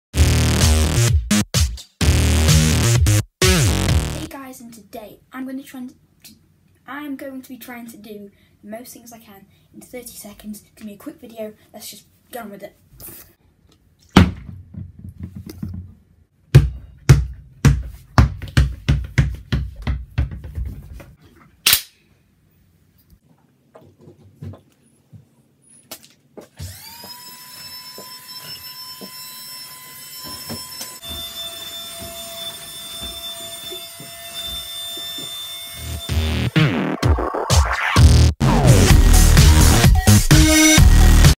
The things I can do in 30 seconds E1 S1
Hey guys and today I am doing the most things I can in 30 seconds that’s basically it hope you enjoy!